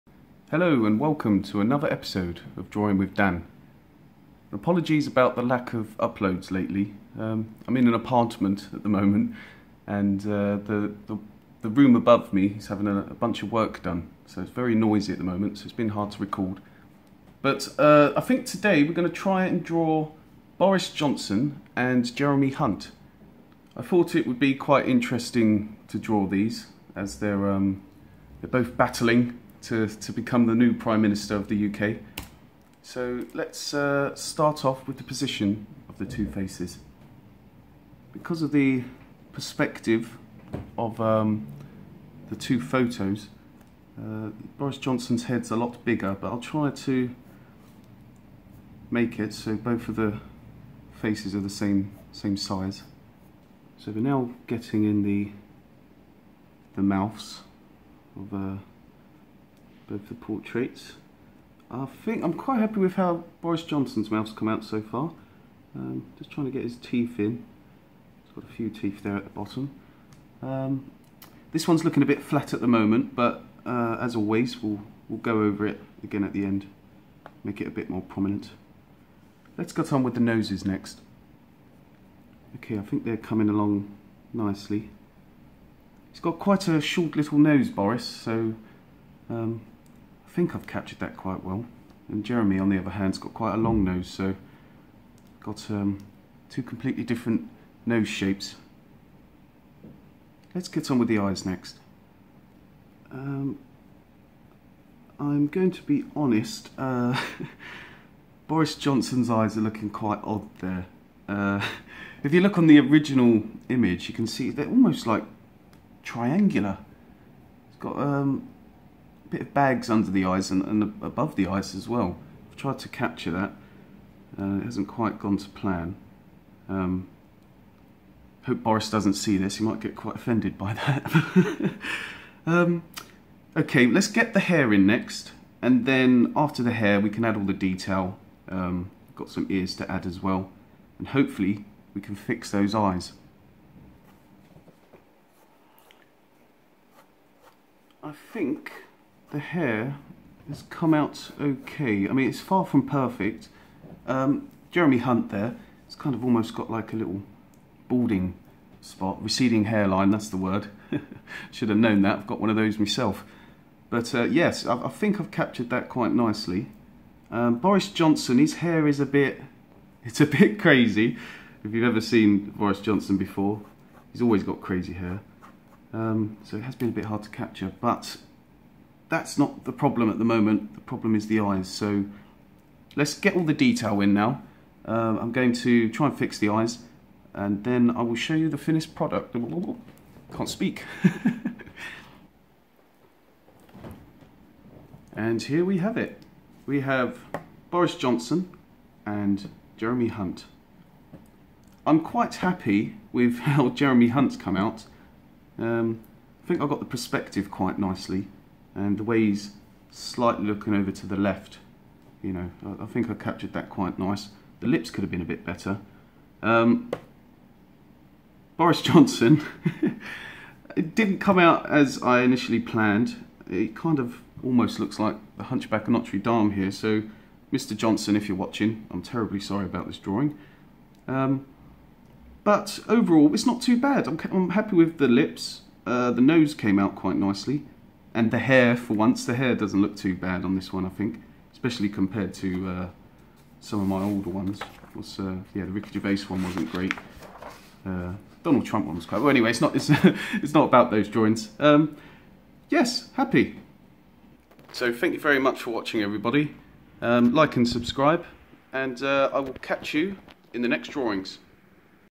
Boris Johnson vs Jeremy Hunt - Prime minister of the UK
Free hand drawing of Boris Johnson and Jeremy Hunt, both competing to become the new Prime minister of the United Kingdom.

drawing videos
drawing tips
drawing art
asmr